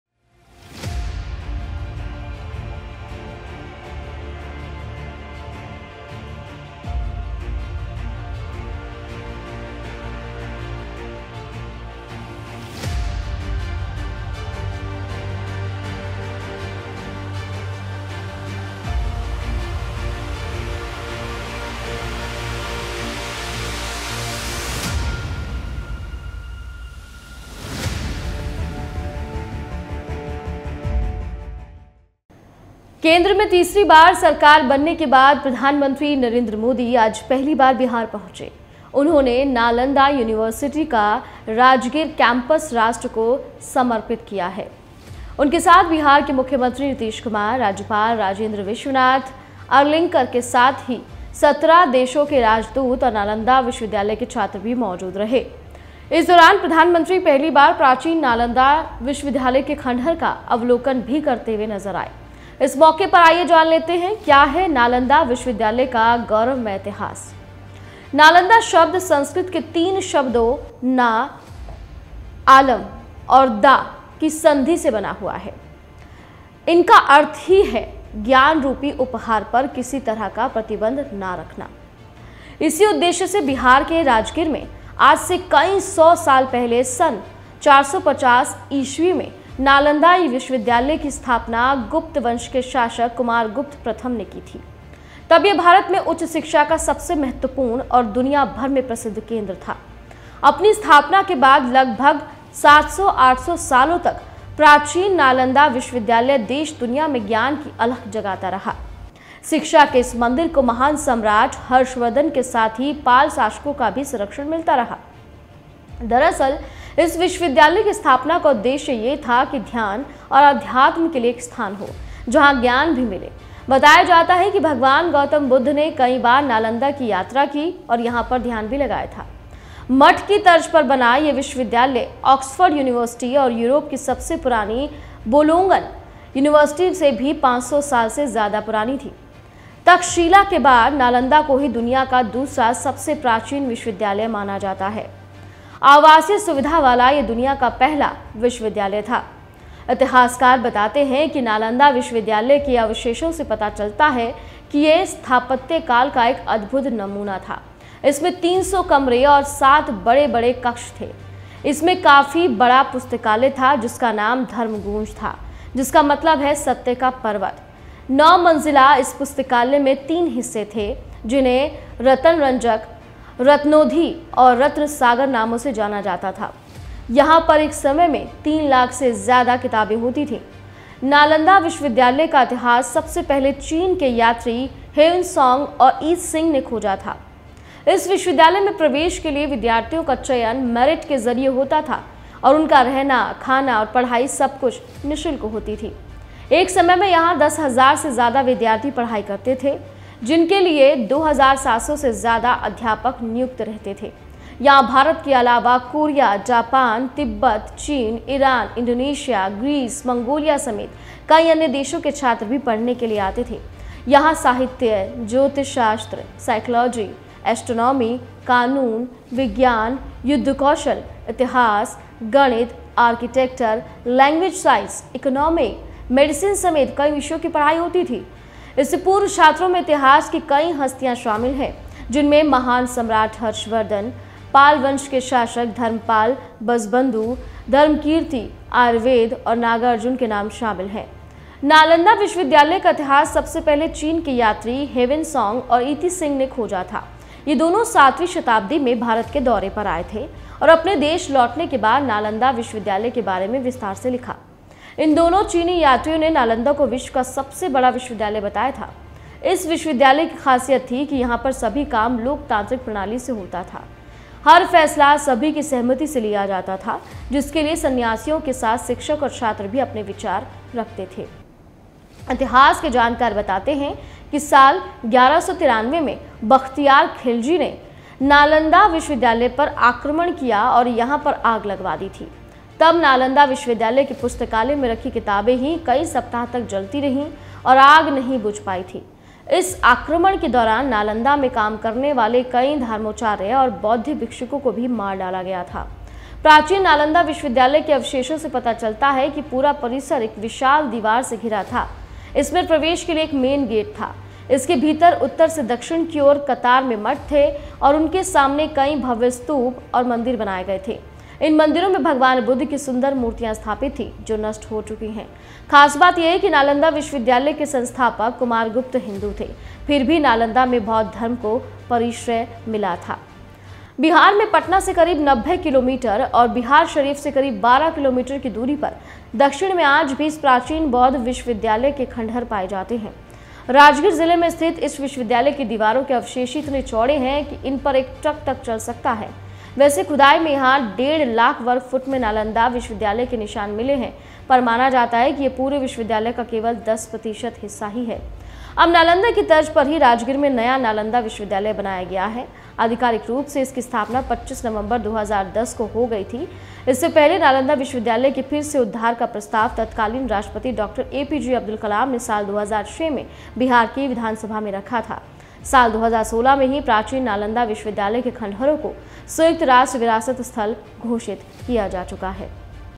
पीएम मोदी ने नालंदा यूनिवर्सिटी को राष्ट्र को किया समर्पित | NALANDA | UNIVERSITY
After forming the government at the Center for the third time, Prime Minister Narendra Modi reached Bihar for the first time today. He dedicated the Rajgir campus of Nalanda University to the nation. Along with him, Bihar Chief Minister Nitish Kumar, Governor Rajendra Vishwanath Arlenkar, ambassadors of 17 countries and students of Nalanda University were also present.